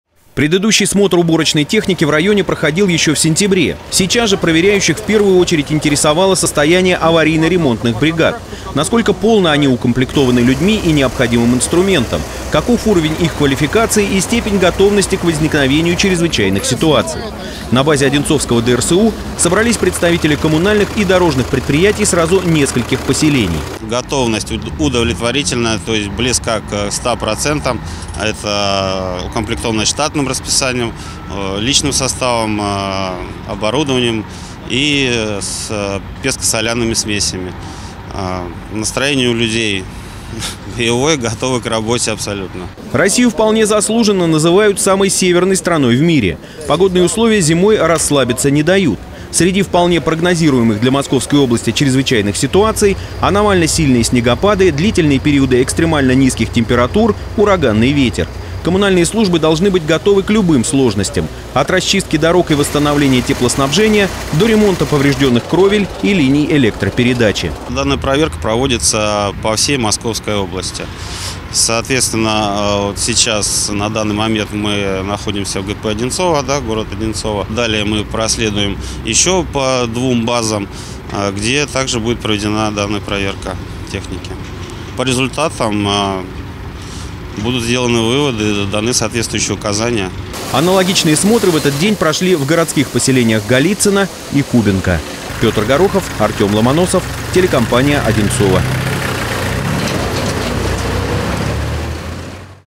На базе Одинцовского ДРСУ прошла проверка готовности уборочной техники и аварийных бригад к предстоя
Смотр коммунальным и дорожным предприятиям устроили представители районной администрации, поселений и Государственного административно-технического надзора. Насколько готовы в Одинцово к трудностям зимнего периода, выяснял Пётр Горохов.